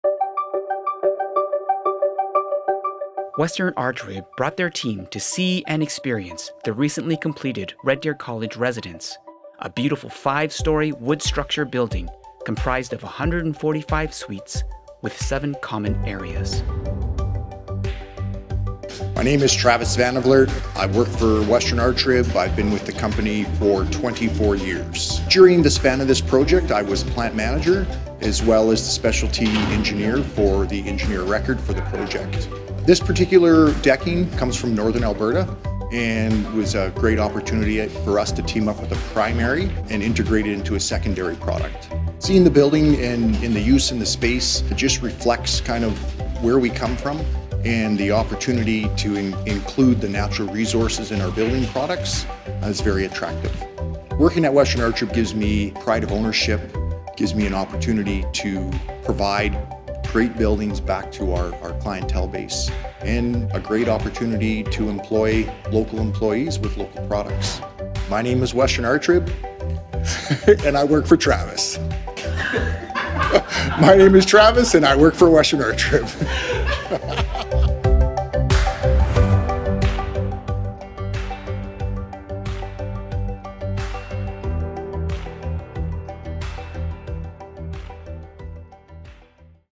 Red Deer College Residence
Interview with Travis Van De Vliert (General Manager of Western Archrib) on the construction of the Red Deer College Residence